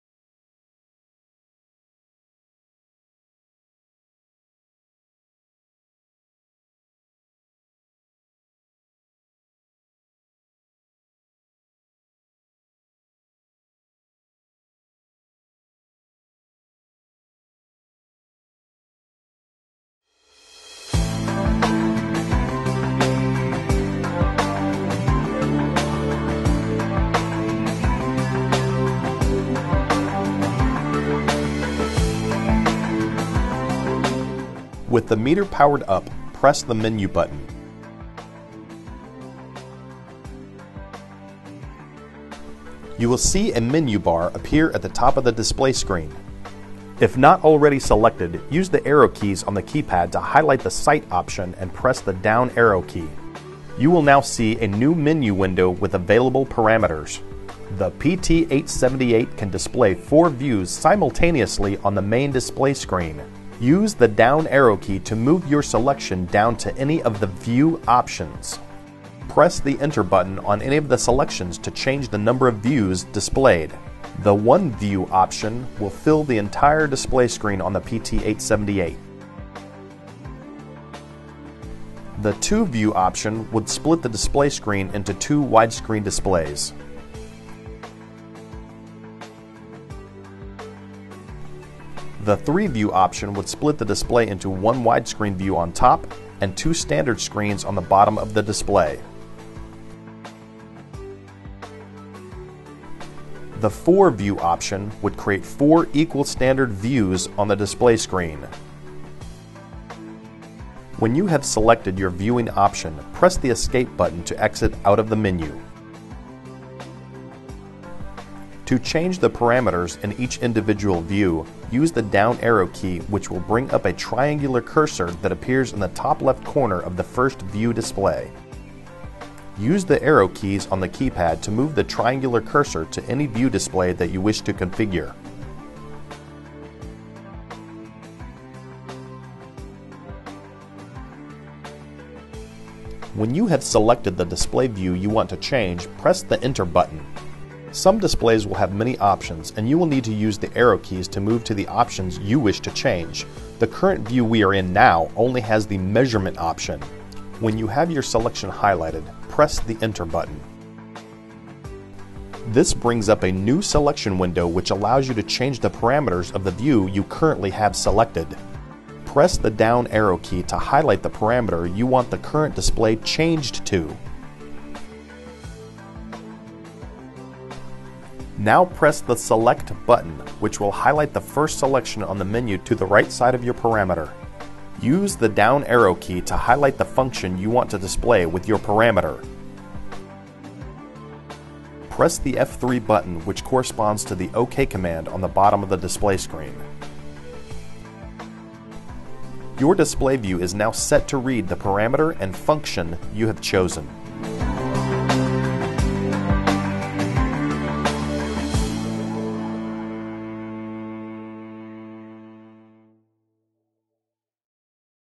PT878 - How to change display viewing and parameters
How to select multiple display viewing and change display parameters on the PT878.  Overview and demonstration of how to select multiple display viewing and change display parameters on the PT878 flow meter.